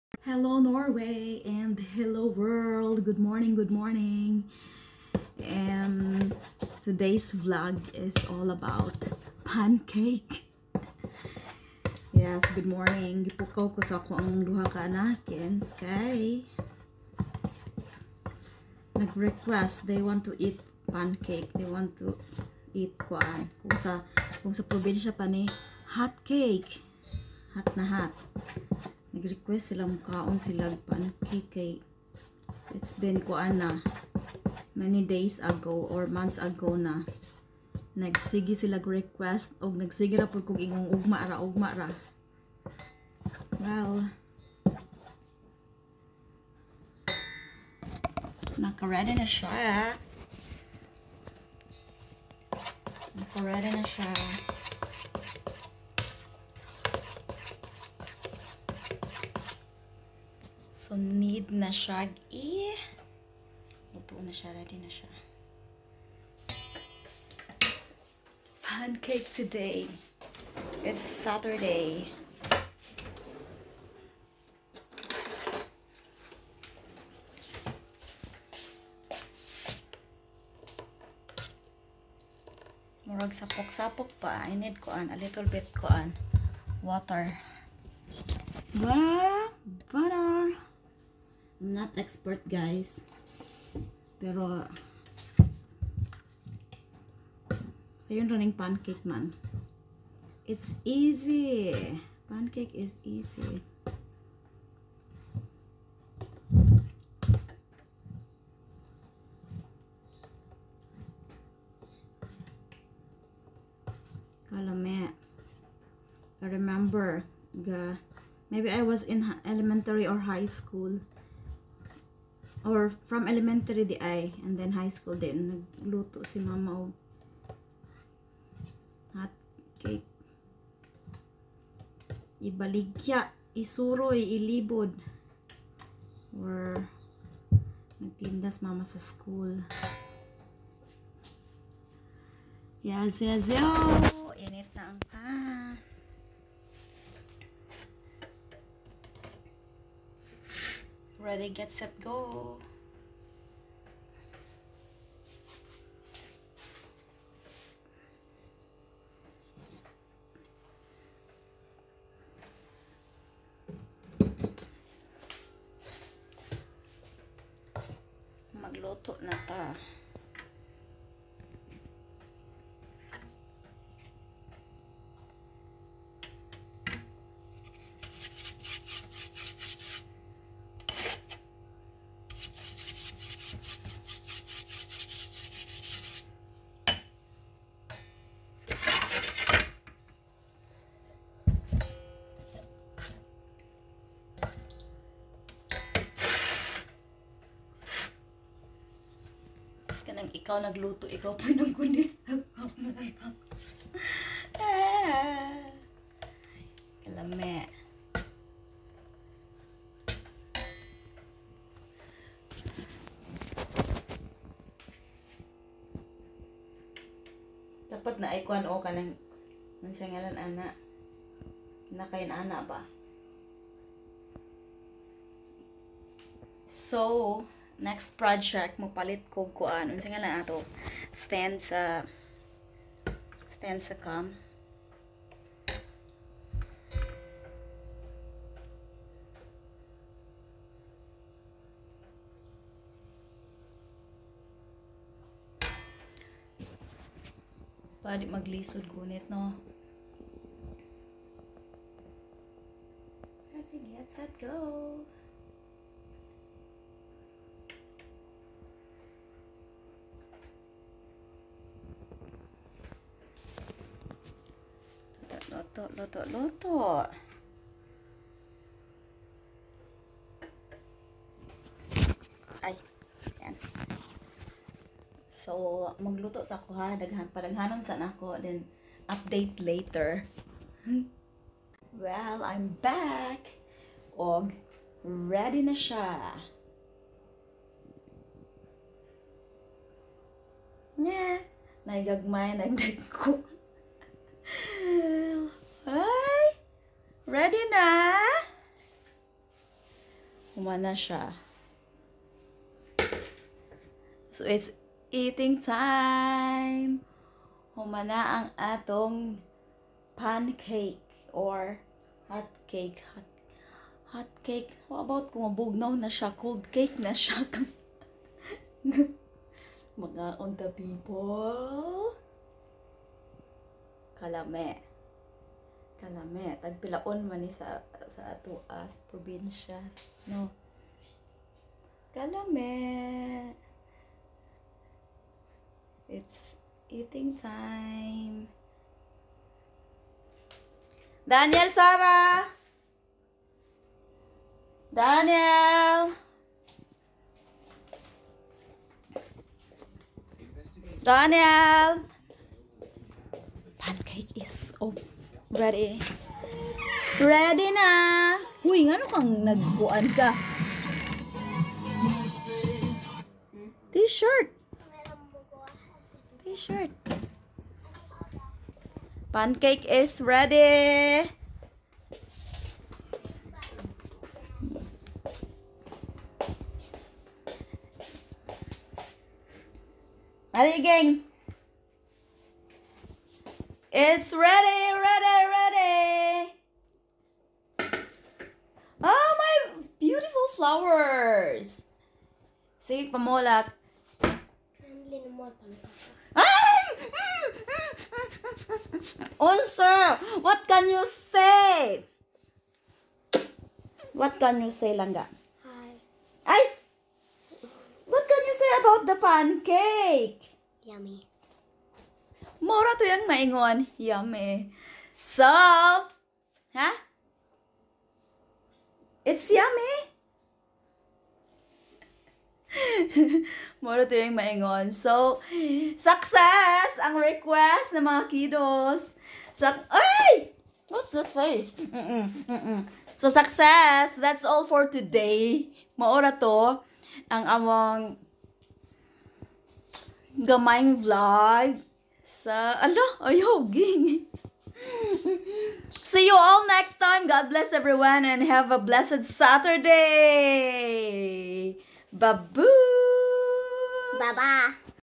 Its Saturday | request granted by kiddos
cooking their favorate pancake